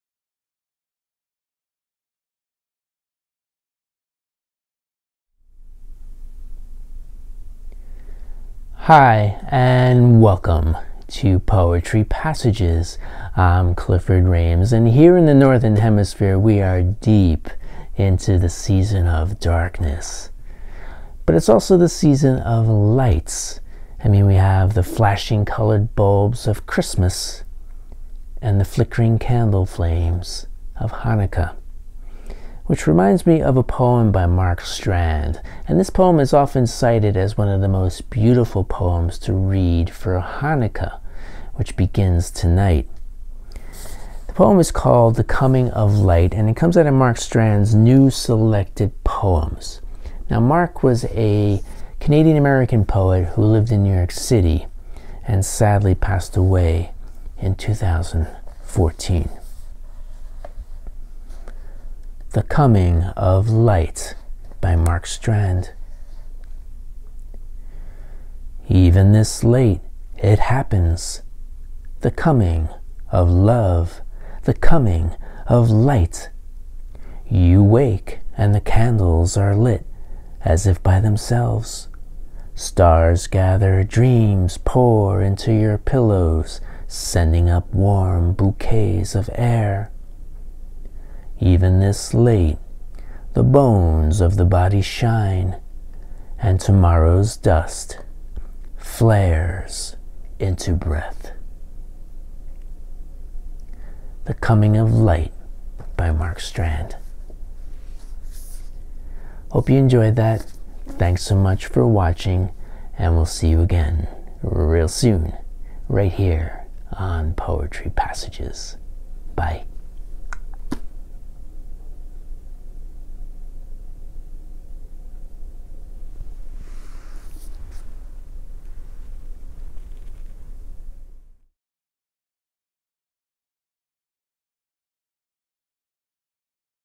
Poetry Passages 153: "The Coming of Light" by Mark Strand
"The Coming of Light" by Mark Strand (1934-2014), from New Selected Poems (Alfred A. Knopf; 2007).